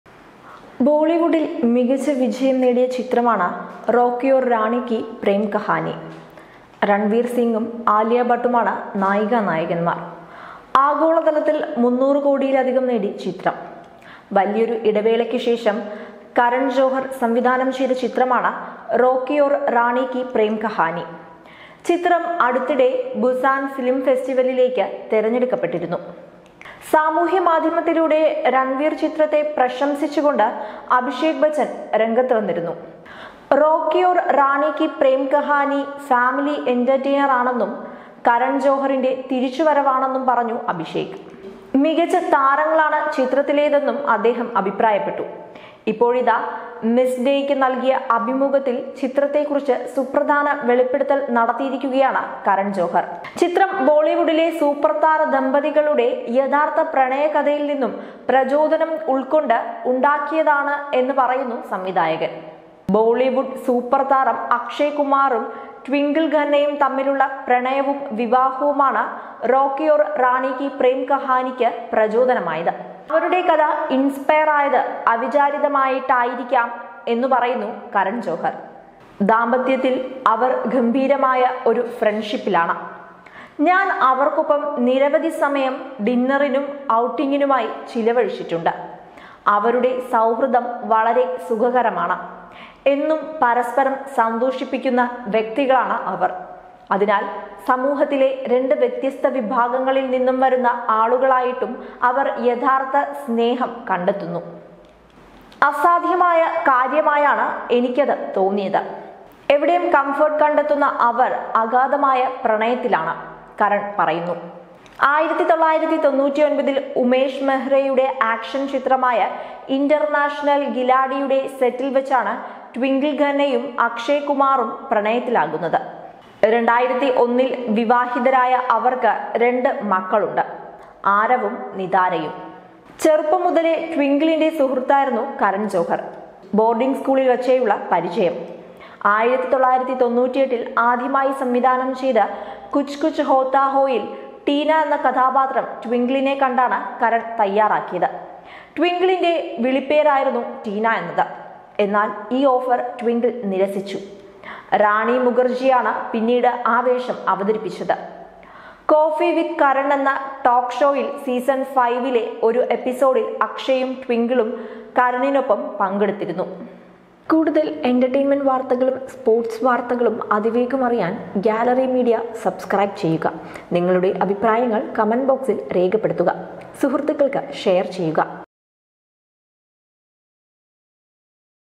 റോക്കി ഓര്‍ റാണി കീ പ്രേം കഹാനി ബോളിവുഡ് താരങ്ങളുടെ പ്രണയ കഥ
ബോളിവുഡിൽ മികച്ച വിജയം നേടിയ ചിത്രമാണ് റോക്കി ഓര്‍ റാണി കീ പ്രേം കഹാനി. രണ്‍വീര്‍ സിംഗും ആലിയ ഭട്ടുമാണ് നായിക നായകന്മാർ. ആഗോളതലത്തില്‍ 300 കോടിയലധികം നേടി ചിത്രം. വലിയൊരു ഇടവേളയ്ക്ക് ശേഷം കരണ്‍ ജോഹര്‍ സംവിധാനം ചെയ്‍ത ചിത്രമാണ് 'റോക്കി ഓര്‍ റാണി കീ പ്രേം കഹാനി'. ചിത്രം അടുത്തിടെ ബുസാൻ ഫിലിം ഫെസ്റ്റിവലിലേക്ക് തെരഞ്ഞെടുക്കപ്പെട്ടിരുന്നു.